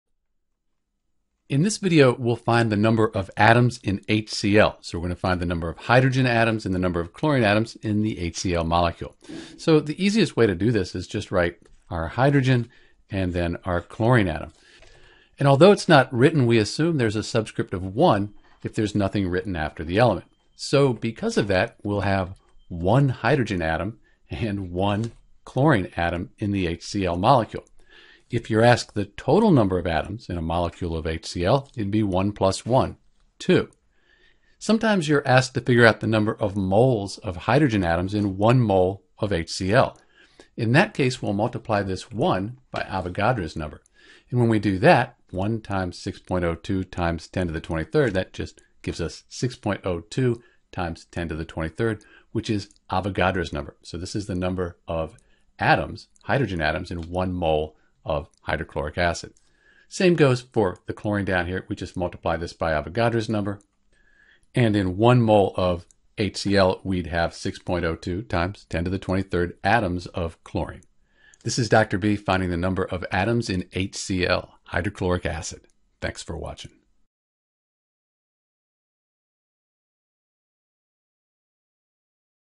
How to Find the Number of Atoms in HCl (Hydrochloric acid)
To find the total number of atoms in HCl (Hydrochloric acid) we’ll add up the number of each type of atom. The small number after the element symbol is called the subscript and this tells us how many of that atom are in the compound. For example, H2 means there are two Hydrogen atoms.

If we add up all of the atoms for HCl we well find that we have a total of 2 atoms for Hydrochloric acid .

This is how to find the number of atoms in Hydrochloric acid (HCl).